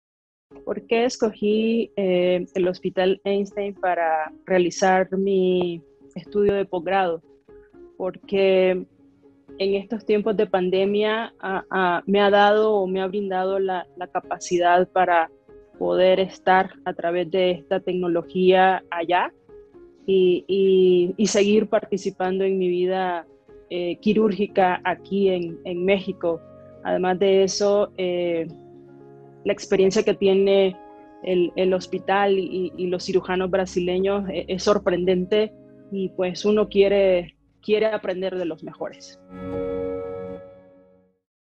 Pós-graduação em Cirurgia Robótica do Einstein - Depoimento Francina Morales
A Francina Morales atua como cirurgiã torácica no México e viu na Pós-graduação do Ensino Einstein uma oportunidade de se atualizar em em Cirurgia Robótica. Assista ao seu depoimento!